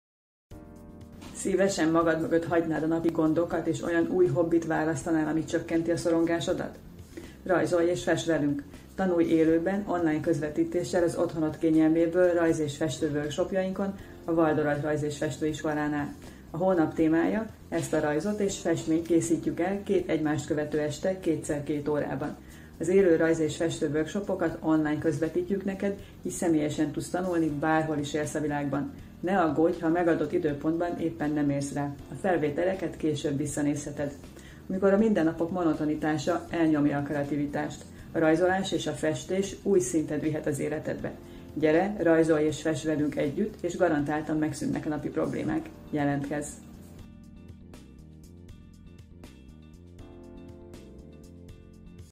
4 órás Rajz és Festő workshopok ÉLŐ KÖZVETÍTÉSSEL!
A képeket két egymást követő este 2x2 órában 19:00-től 21:00-ig készítheted el velünk!

A legközelebbi időpontok:
RAJZ workshop: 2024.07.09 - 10. kedd-szerda, 19:00-21:00
FESTŐ workshop: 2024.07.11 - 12. csütörtök-péntek, 19:00-21:00
Ha nem tudsz jelen lenni, akkor sincs gond, a workshopok felvételeit meg is vásárolhatod, így bármikor visszanézheted!